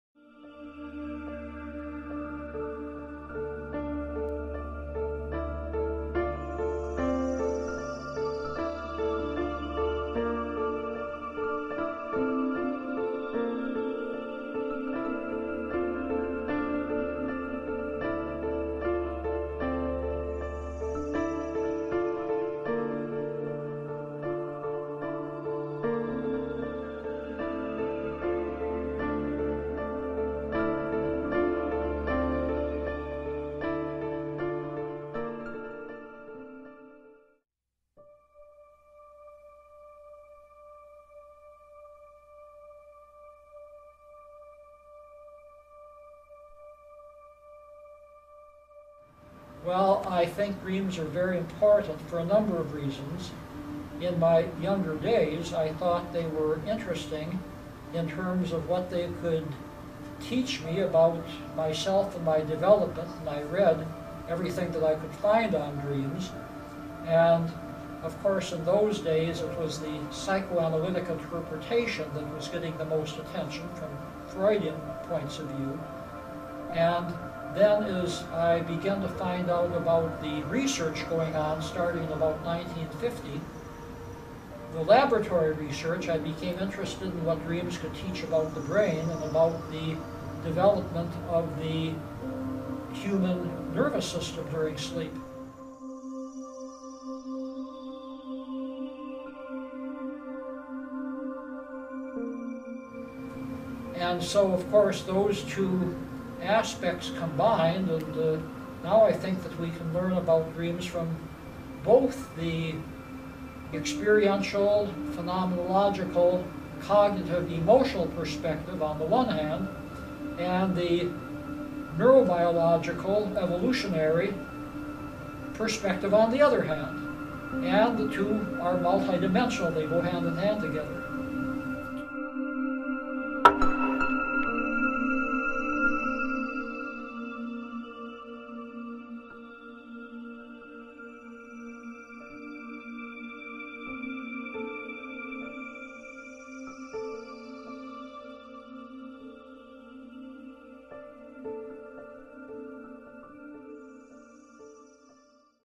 Dream Glimpse Ep.1 (Dream interpretation with Dr. Krippner)
Episode 1 of the Indigo-Chef's original documentary series DREAM GLIMPSES features world-reknowned psychologist Dr. Stanley Krippner of Saybrook Institute in San Francisco.  Join Dr. Krippner and Hezekiah Condron as they discuss YOUR dreams!